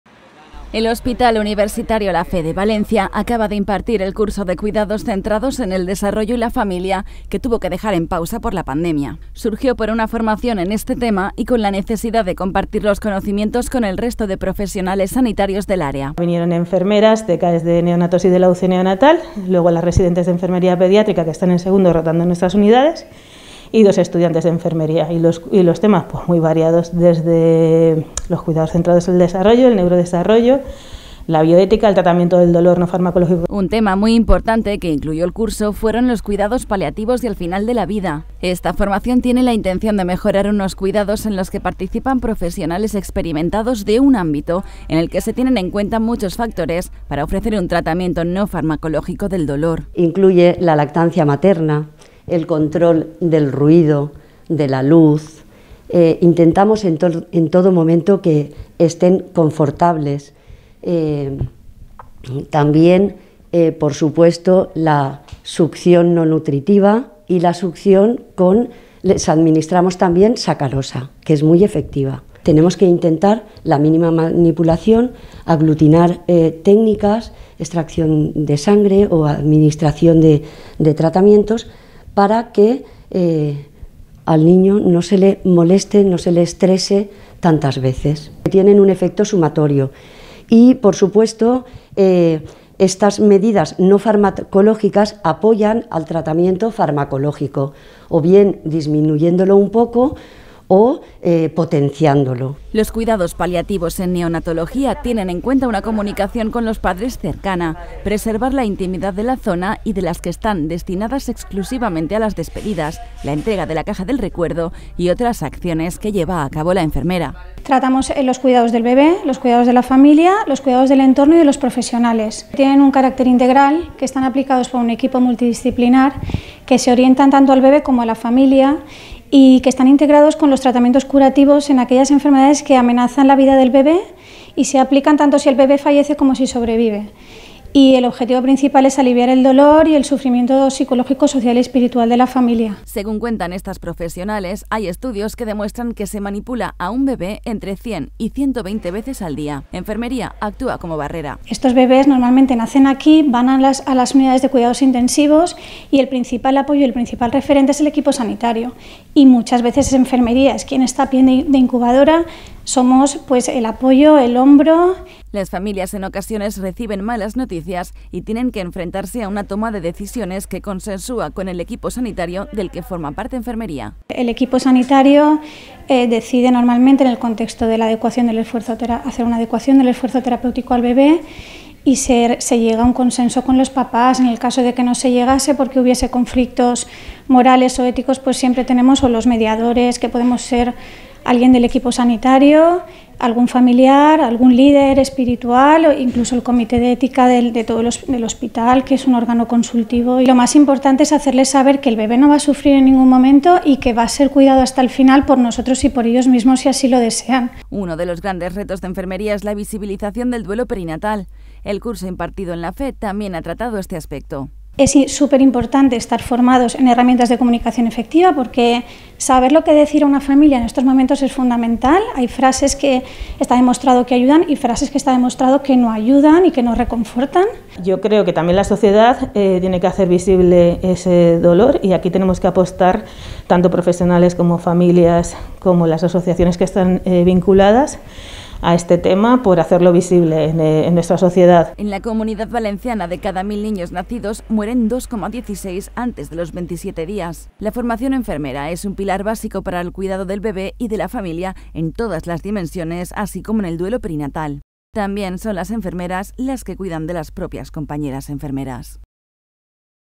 Cuidados Paliativos y al final de la vida | Cecova TV
El Hospital Universtario La Fe de Valencia impartió el curso de Cuidados Centrados en el Desarrollo y la Familia e incluyó un tema en la que cuentan con unos cuidados enfermeros en los que tienen experiencia y reconocimiento, los Cuidados Paliativos y al final de la vida.